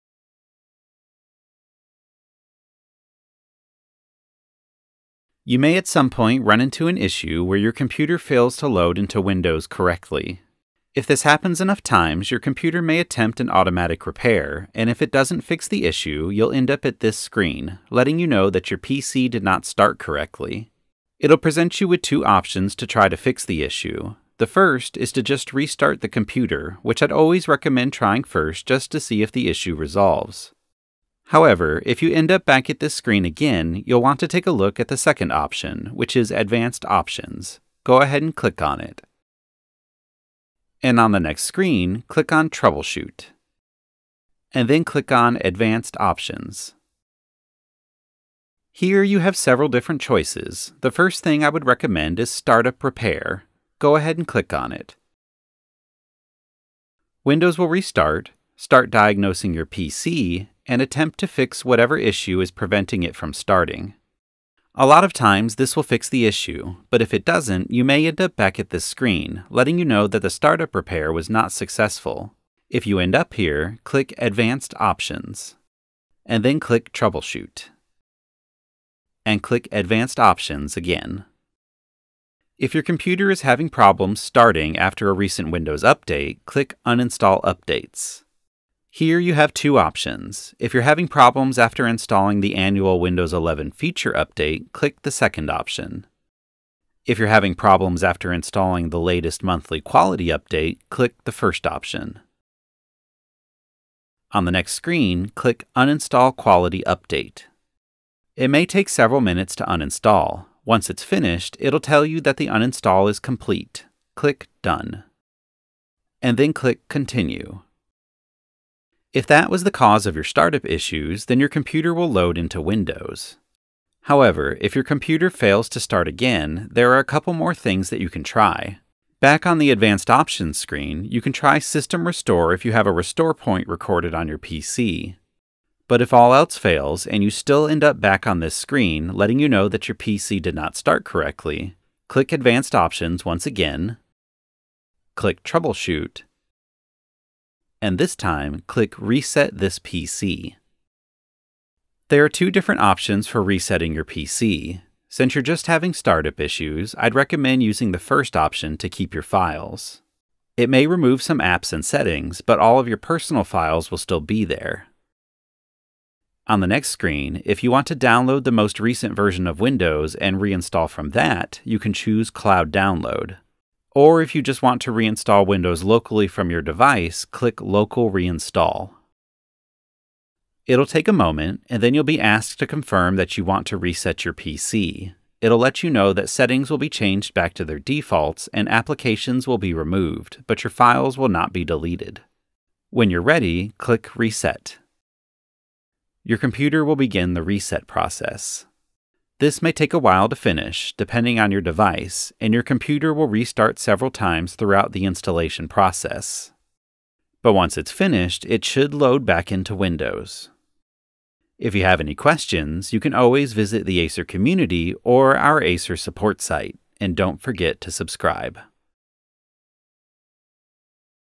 Windows 11 - How to Fix Startup Issues
Startup issues can be the result of unsuccessful updates, hardware failure, or driver problems. If your PC fails to startup multiple times, Windows will automatically prompt you with the Automatic Repair screen.

This video walks you through the different advanced options that Windows offers to resolve startup issues.